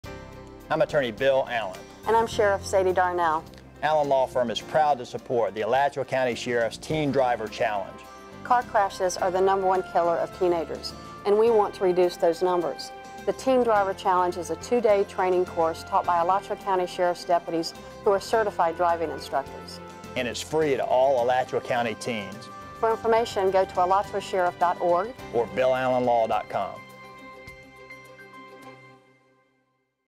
Alachua County Sheriff's Office Teen Driving PSA
Attorney Bill Allen and Alachua County Sheriff Sadie Darnell promote the Teen Driver Challenge.